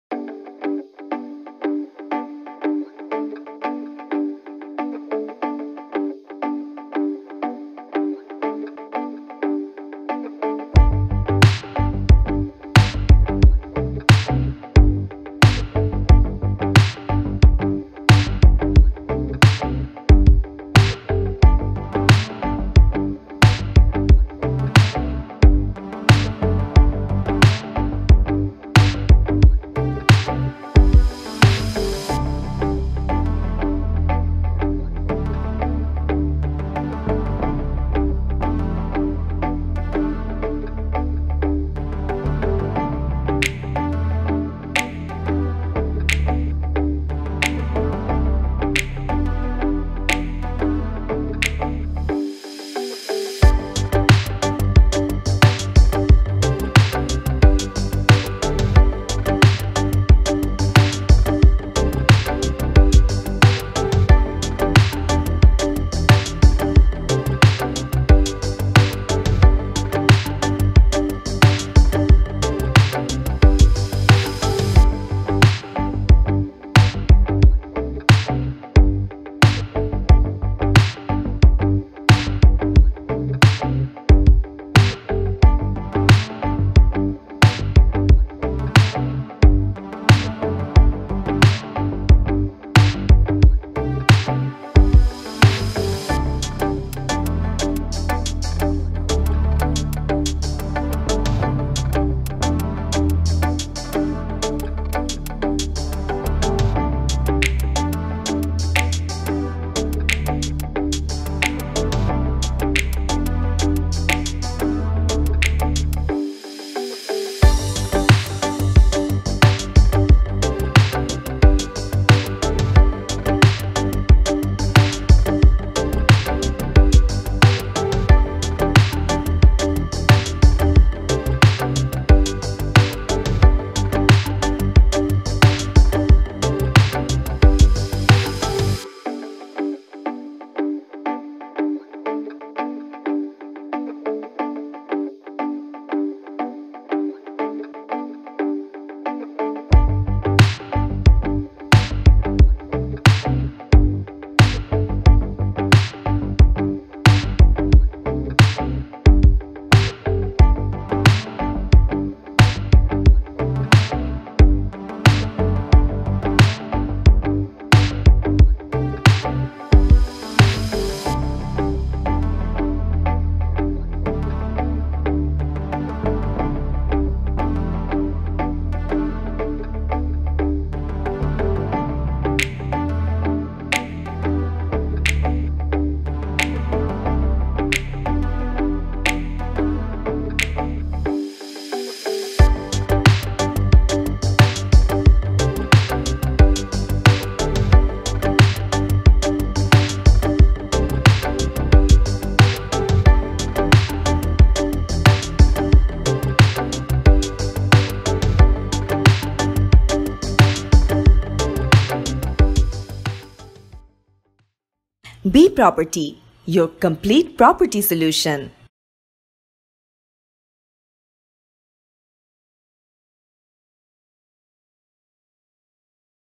Favorable 1,455 sq. ft. Flat in Kafrul | Flat for Sale in Dhaka
This nearly finished flat is located in kafrul which is near to Dhaka-maymansing highway. Kafrul makes sure a content living for your family. There is an opportunity for any buyer to get a brand new apartment. You can bring luxury as your wish.

Bproperty‌ ‌flatSale‌ ‌flatRent‌ ‌Duplex‌ ‌Triplex‌ ‌Fullbuilding‌ ‌Apartment‌ ‌